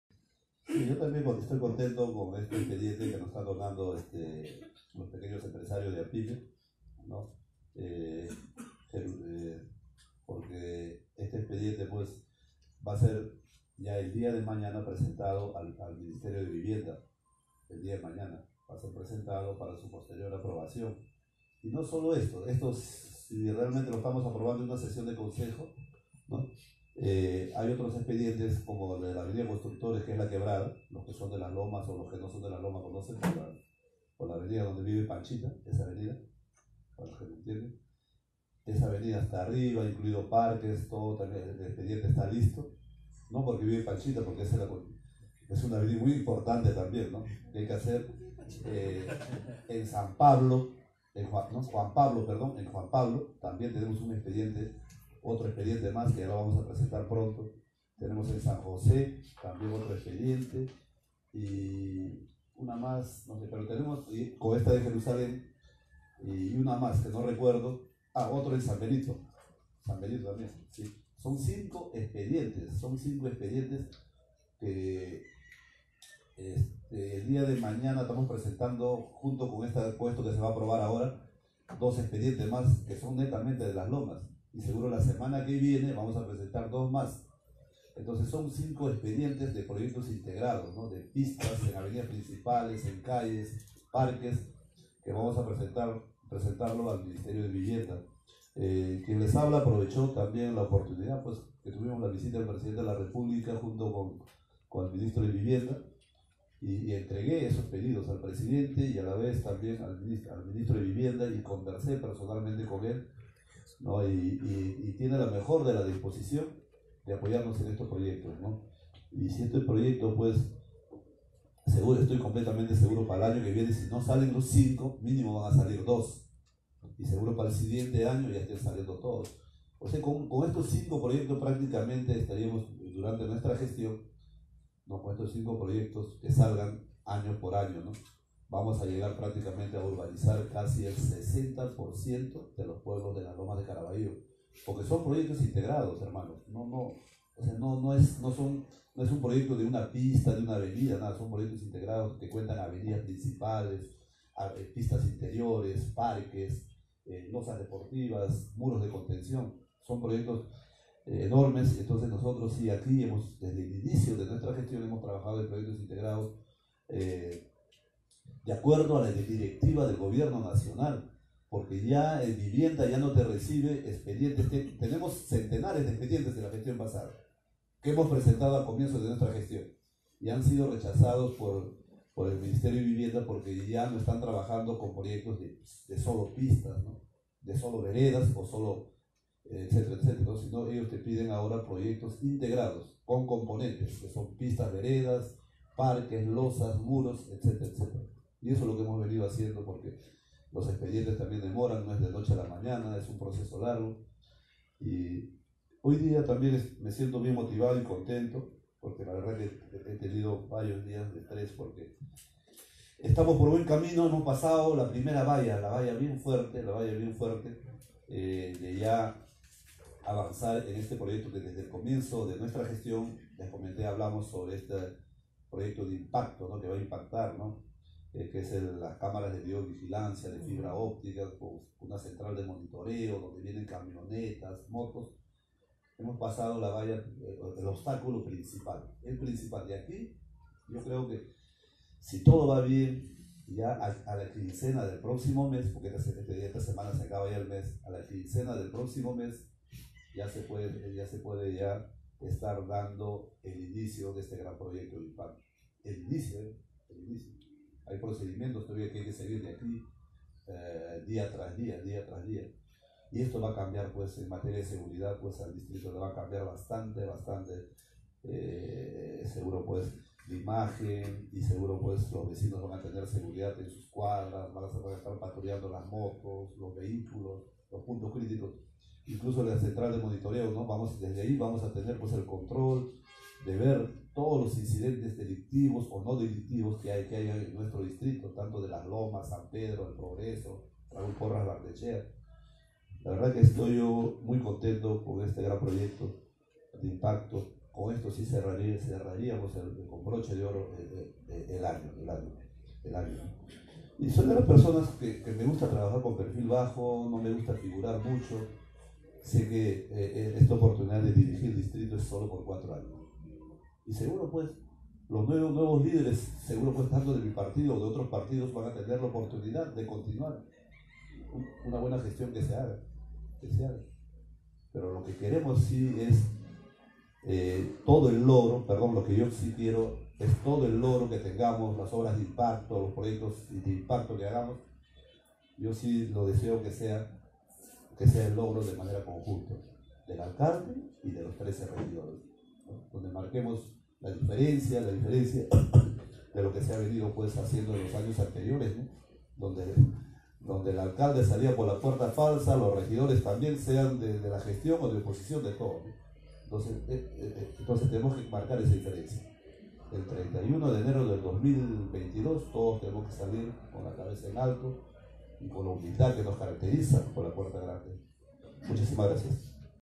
90% de pistas y veredas se ejecutarán en Lomas de Carabayllo, anunció alcalde Marcos Espinoza Ortiz
De otro lado, dijo que la infraestructura de seguridad ciudadana se potenciará con nuevas unidades vehiculares y tecnología de punta
En la sesión ordinaria de concejo municipal del pasado martes 24 de setiembre, el alcalde Marcos Espinoza realizó importantes anuncios a su concejo municipal, entre ellos dijo que en su periodo 2019 – 2022 impulsará la ejecución de pistas y veredas en el sector de Lomas de Carabayllo, de tal manera que al concluir su mandato el 90% de la infraestructura urbana de esa parte del distrito este consolidada.
De otro lado, anunció con satisfacción que en los próximos días se lanzará la convocatoria para la adquisición de unidades motorizadas (camionetas, motos bicicletas), cámaras vídeo vigilancia mejorando la infraestructura de la seguridad ciudadana del distrito.
Pidió y agradeció a su concejo municipal el apoyo que le brinda para aprobar las normas que viabilizan el logro de las metas trazadas, invocando a todos que al 30 de diciembre 2022 cuando se entregue el cargo, salgan por la puerta grande y la cabeza levantada, marcando la diferencia de las dos gestiones anteriores, que no tuvieron un final feliz, precisó.